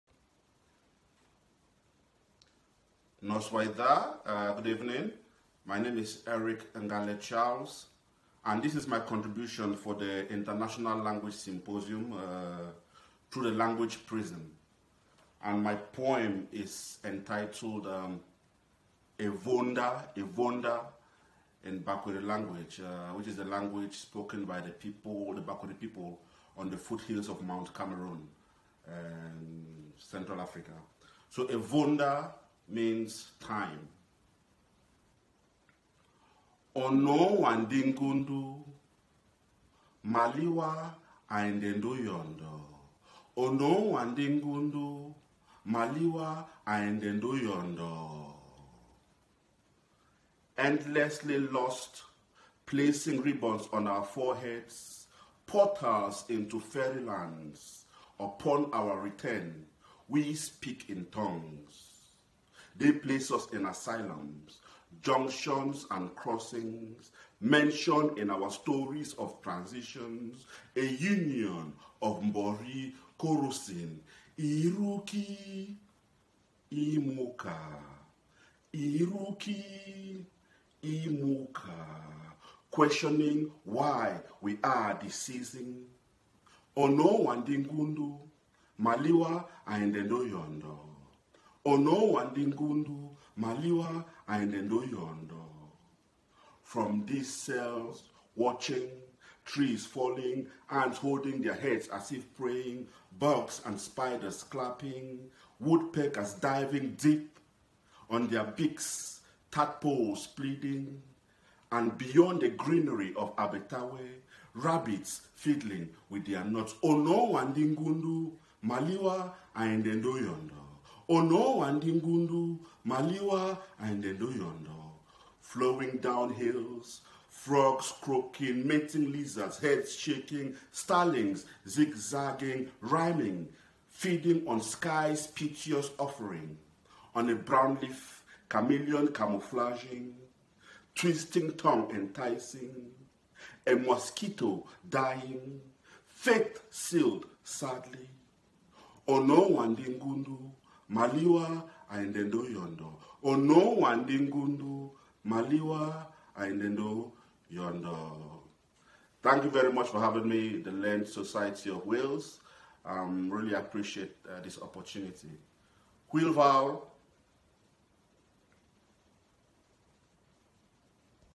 Eric Ngalle Charles: 'Evonda'
Watch Camoeroonian-born, Welsh-based poet Eric Ngalle Charles read his poem, 'Evonda', commissioned for our bilingualism event, Through the Language Prism, which takes place next week.

Dyma'r bardd Eric Ngalle Charles yn darllen ei cherdd a gomisiynwyd ar gyfer ein symposiwm ar ddwyieithrwydd, Trwy Brism Iaith, sy'n cael ei gynnal wythnos nesaf. Cofrestrwch nawr. Manylion llawn yma: